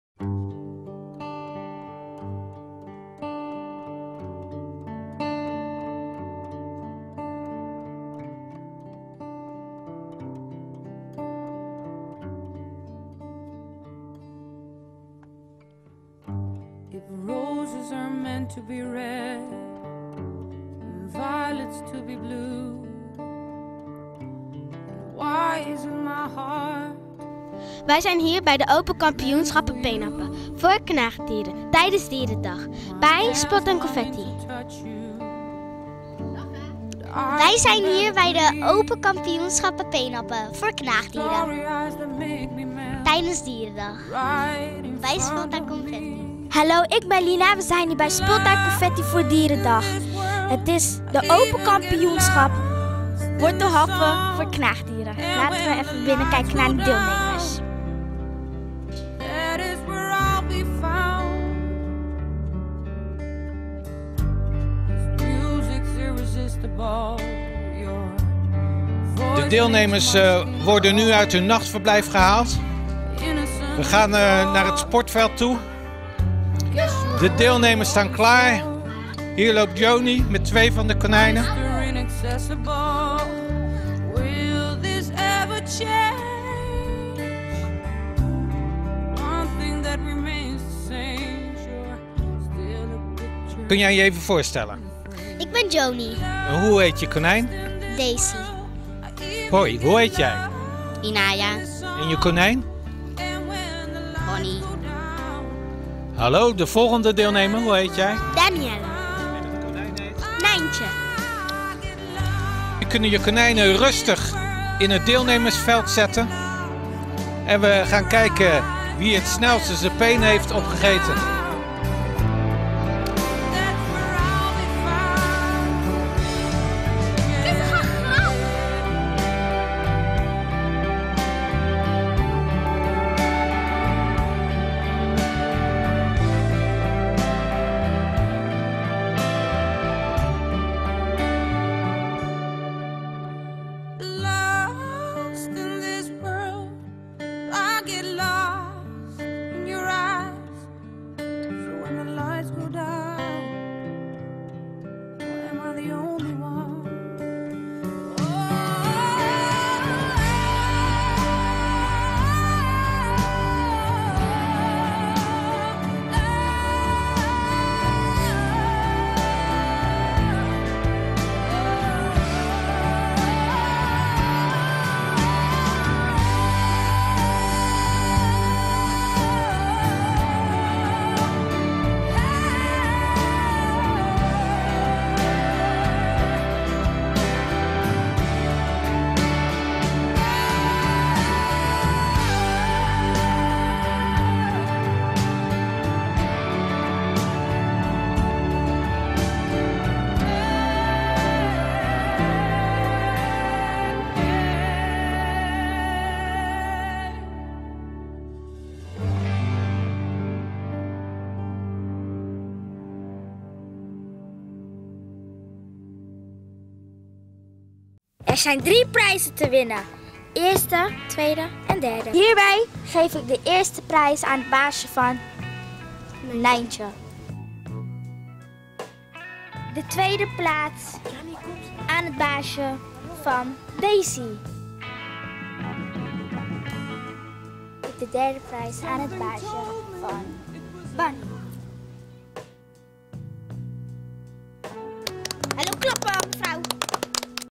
Cineac Delfshaven @ Peen Happen bij Speeltuin Confetti
Tijdens Dierendag wordt er peen gehapt bij Speeltuin Confetti